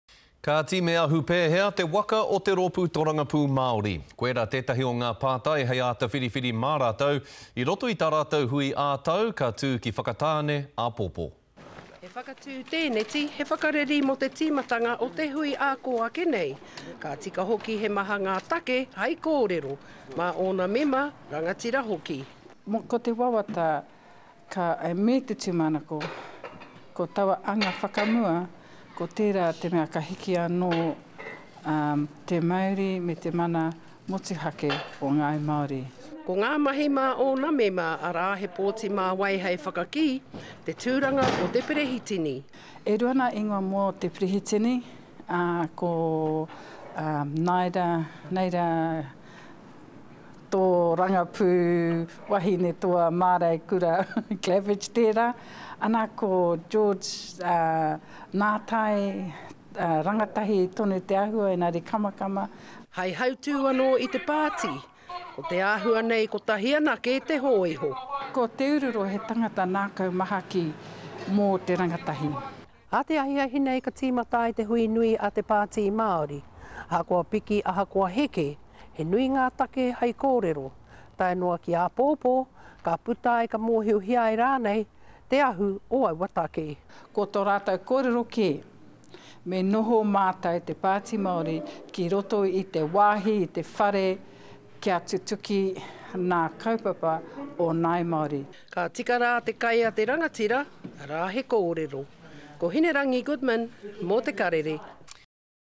Maori Party prepare to vote on presidency at AGM
Where is the Maori Party headed? That's what they'll figure out during their AGM in Whakatane.